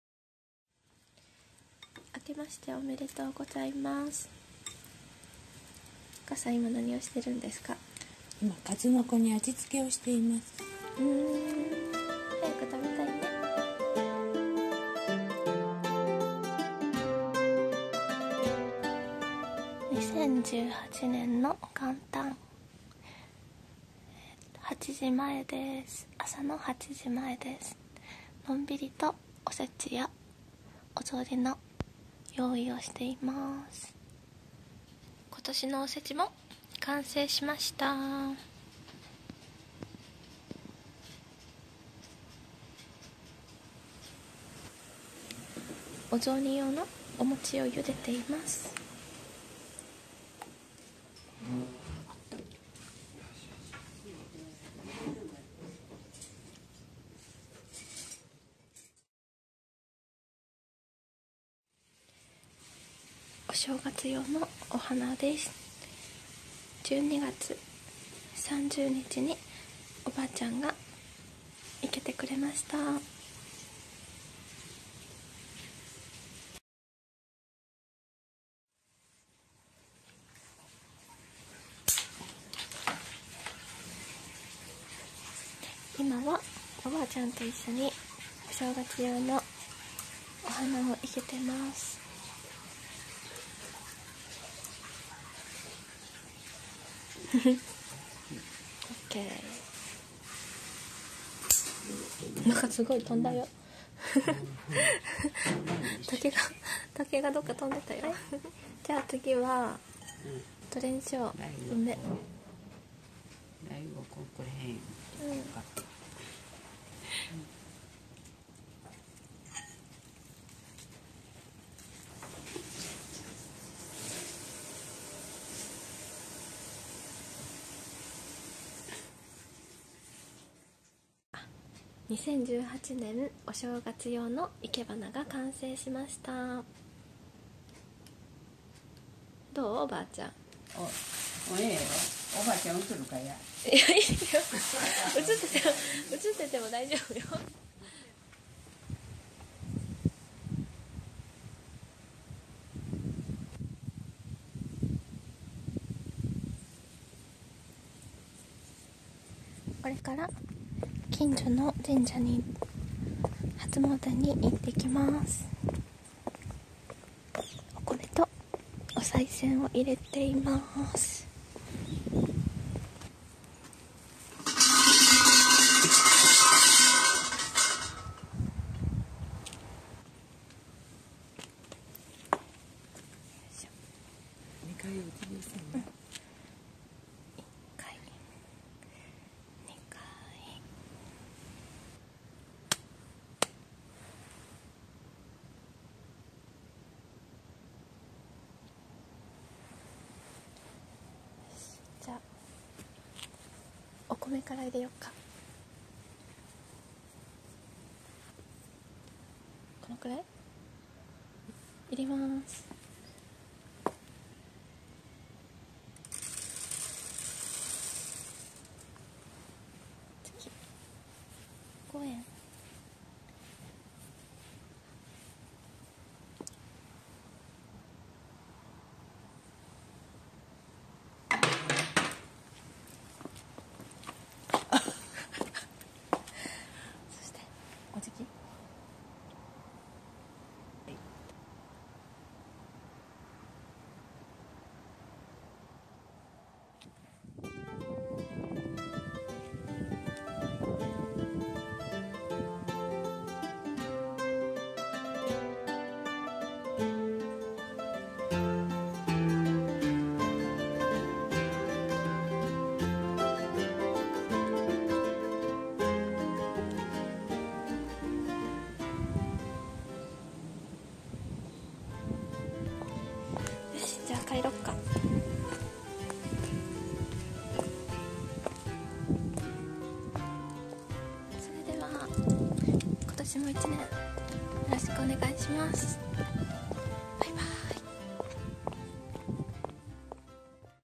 117★short video32★暮らしの歳時記5[2018年]～お正月vlog～[Japanese traditional culture]
2018年★初投稿です＾＾新年明けましておめでとうございます！今年もどうぞよろしくお願いいたします＾＾
【次回予告】仕事多忙のため動画投稿遅れます(/´△`＼)